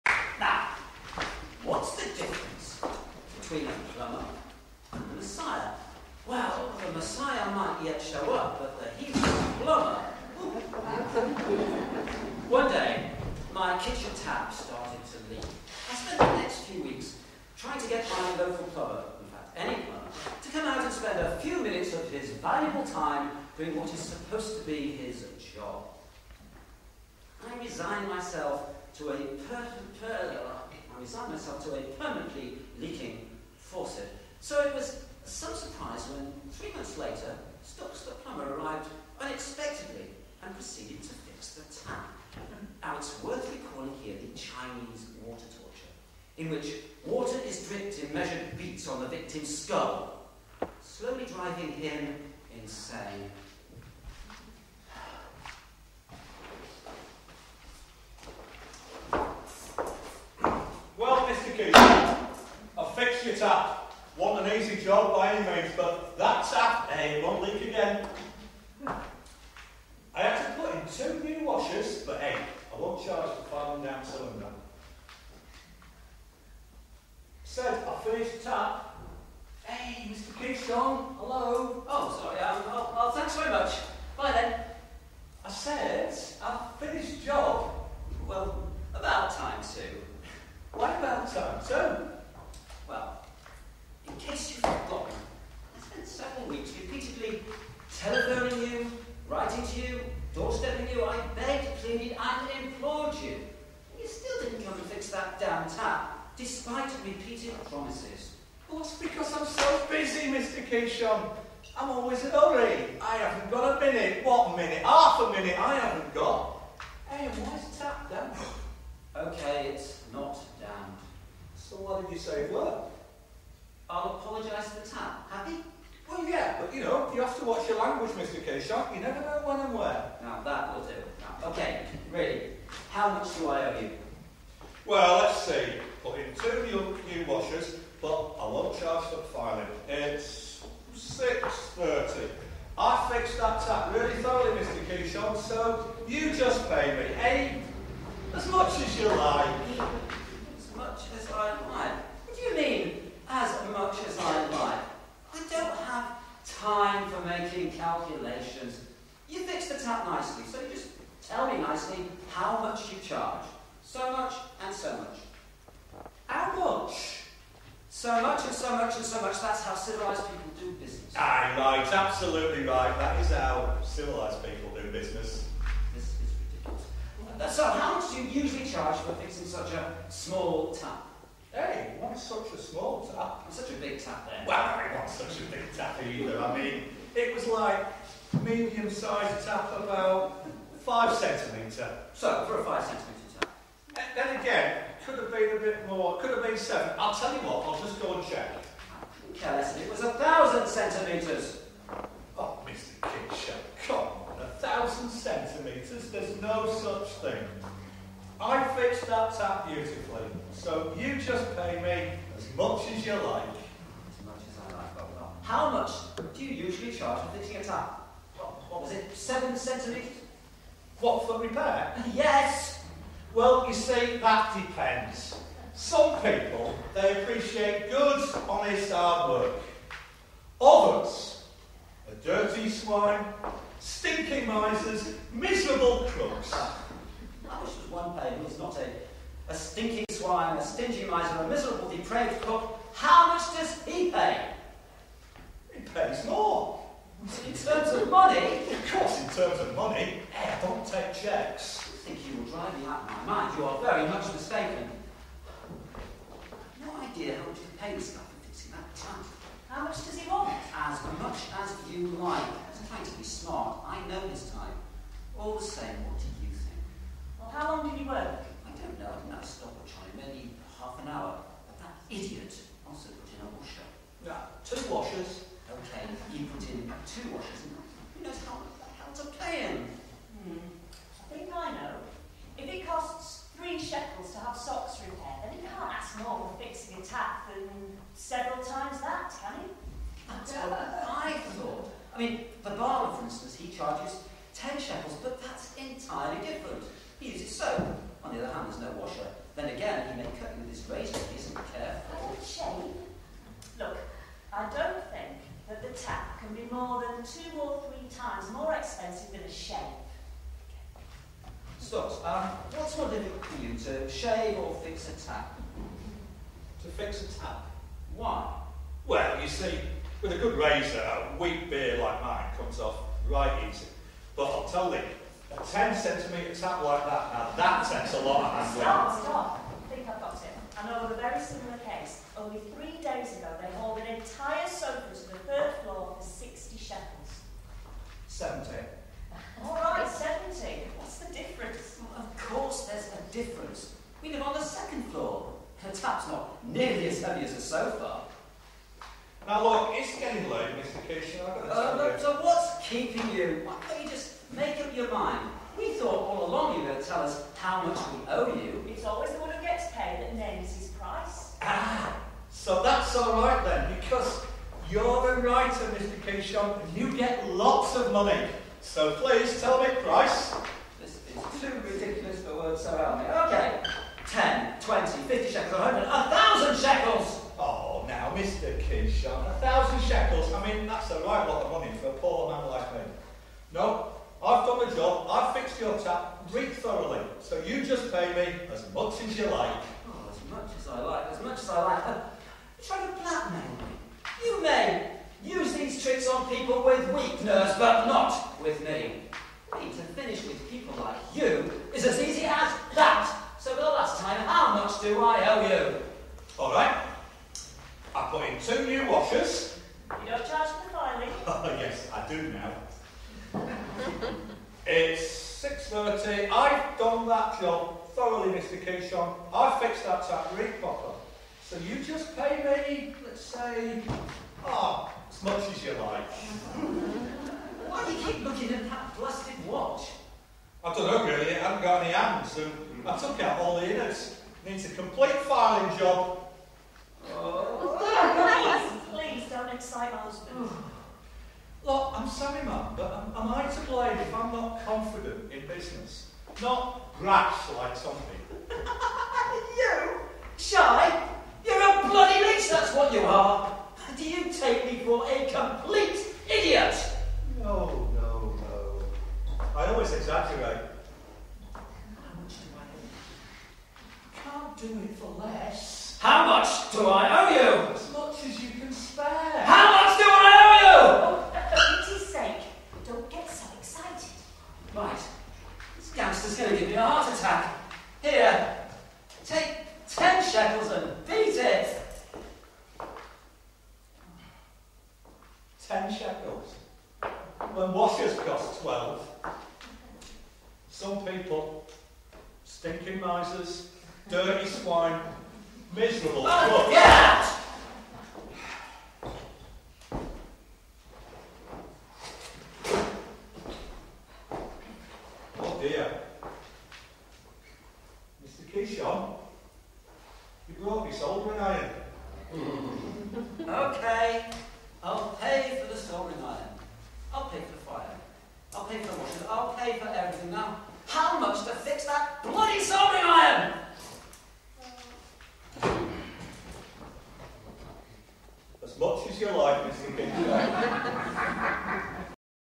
"Jewish Poker": As Much As You Like It
'As Much As You Like It' from Kishon Sink Drama's adaptation of short stories by Ephraim Kishon. "Jewish Poker" was filmed at Otley Courthouse on 16th July 2005. If Shakespeare had ever need an Israeli plumber...